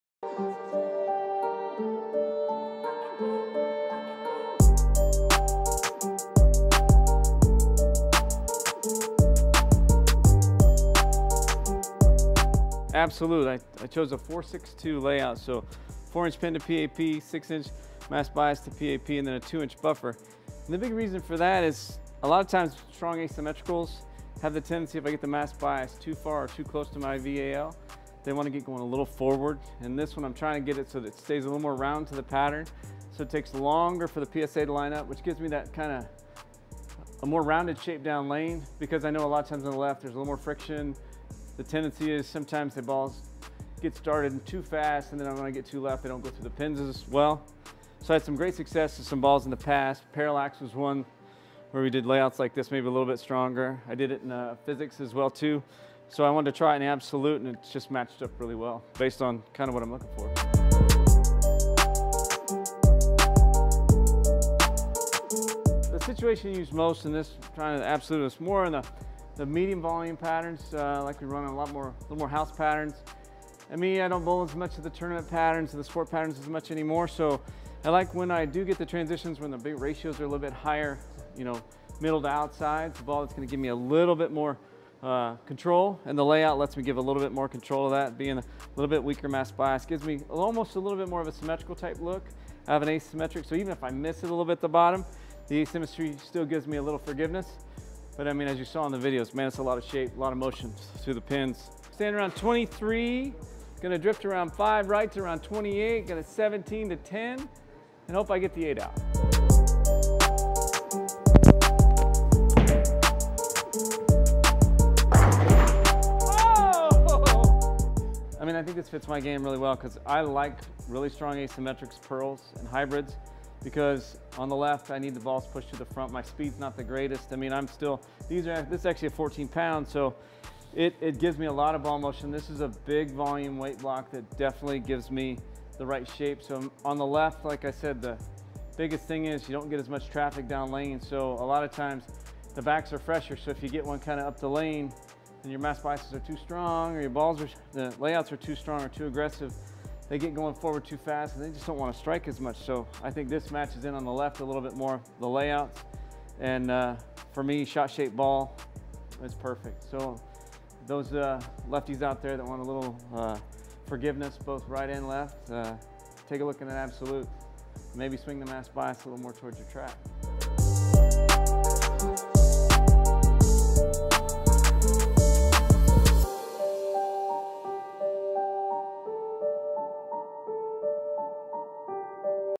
Storm Bowling｜Absolute Bowling Ball Xplained With Hank Boomershine
Hank Boomershine, VP of R&D and Sales, explains why Absolute is the perfect fit for his game.